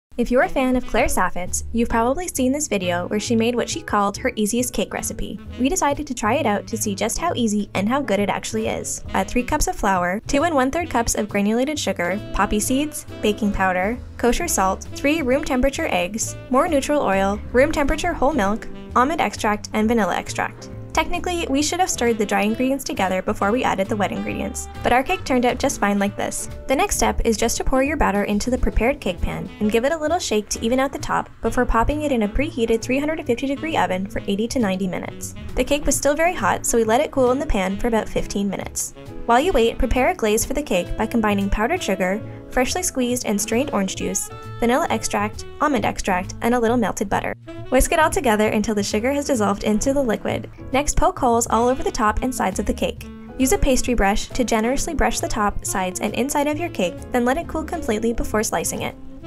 Making The Easiest Cake Recipe - Dished
On today's Dished we're making the Easiest Cake Recipe!

Ingredients for making the easiest cake:
Cake ingredients:
Neutral oil and all-purpose flour for the pan
3 cups all-purpose flour (13.8oz / 390g)
2 1/3 cups sugar (16.4 oz/ 465g)
2 tbsp poppy seeds (0.6 oz / 17g)
1 1/2 tsp baking powder (0.21 oz / 6g)
1 tsp kosher salt (0.11 oz / 3g)
3 large eggs (4.3 oz / 150g)
1 1/3 cups neutral oil, such as vegetable or grapeseed (10.2 oz/ 288g)
1 1/2 cups whole milk (12.7oz / 360g)
1 1/2 tsp vanilla extract
1 1/2 tsp almond extract